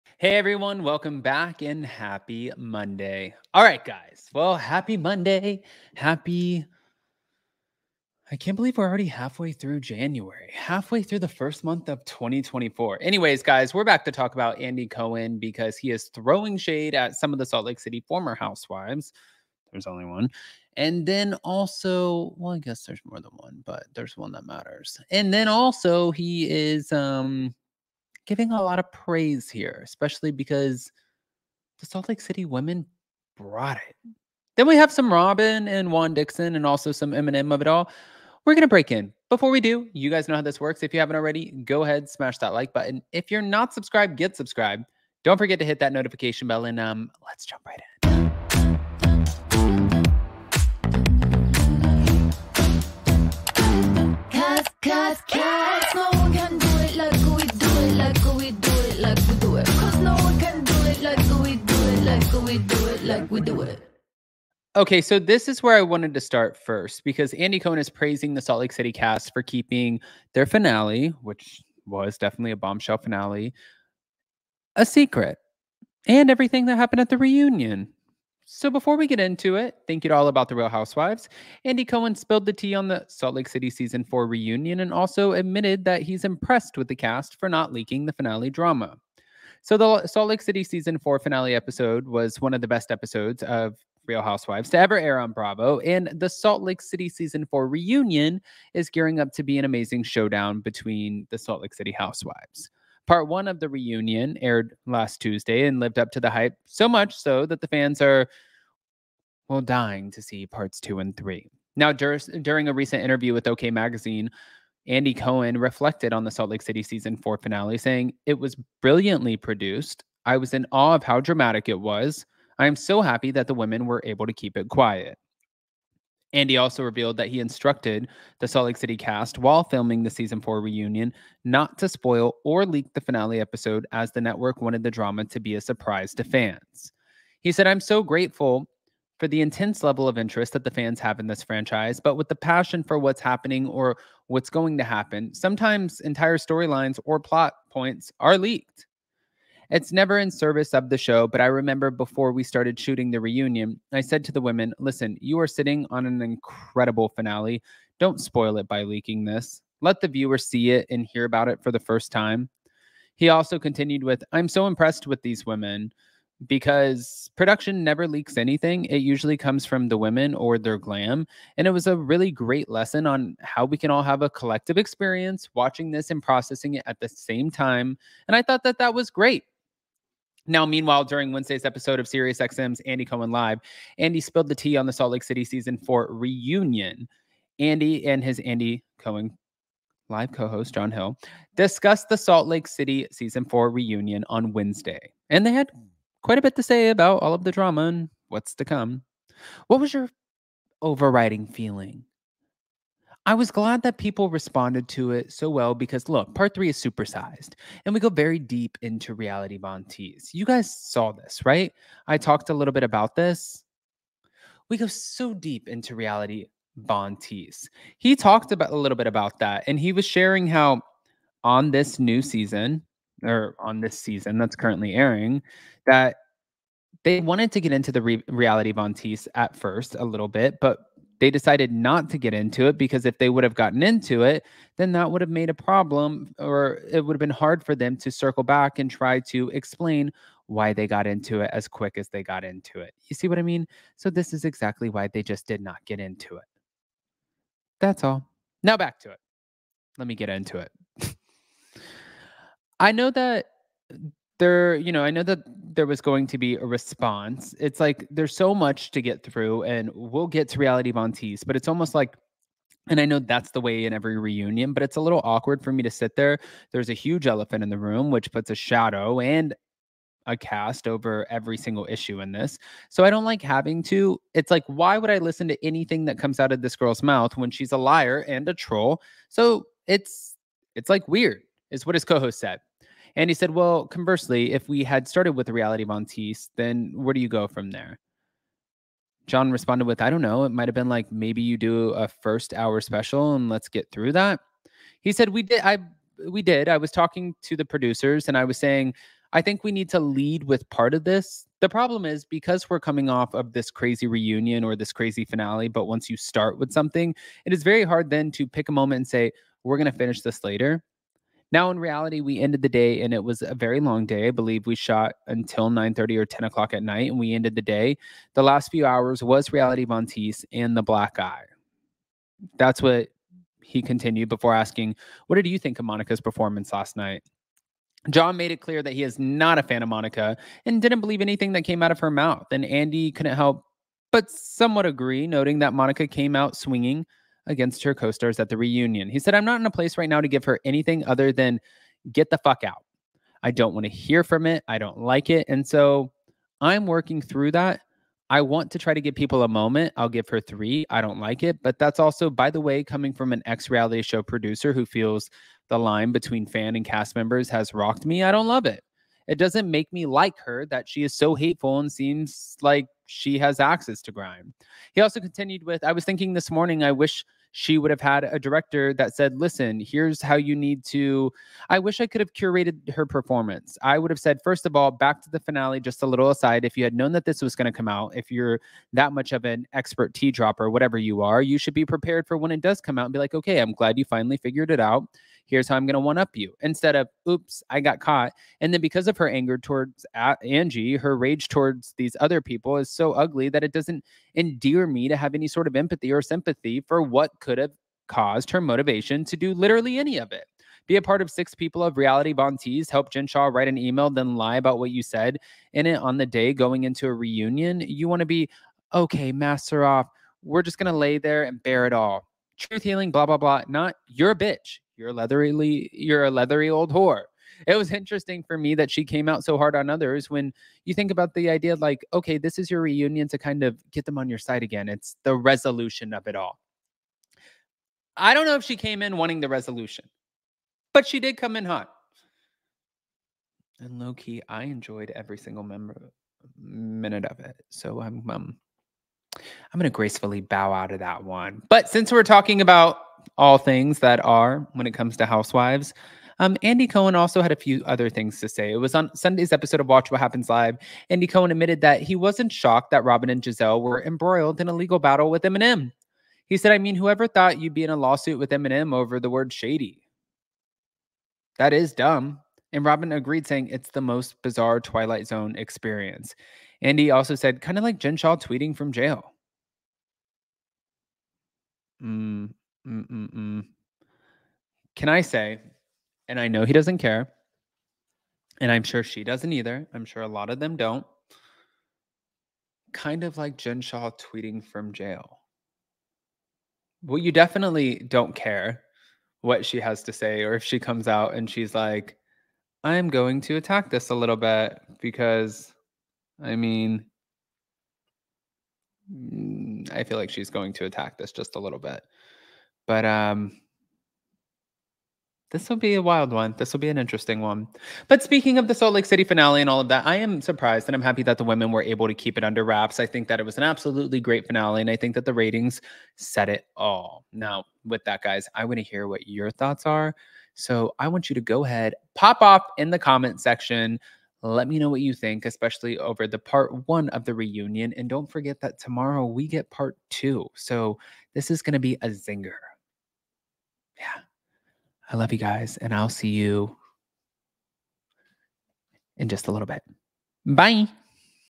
Andy Cohen Is Shocked By The RHOSLC Cast + Entire Cast Returning?
❄️ More RHOSLC Reunion tea leaked before the new episode airs + audio! 👀

🎧 Rather listen?

📬 Mail/Packages:
Up And Adam!
11231 US HWY 1, Suite 257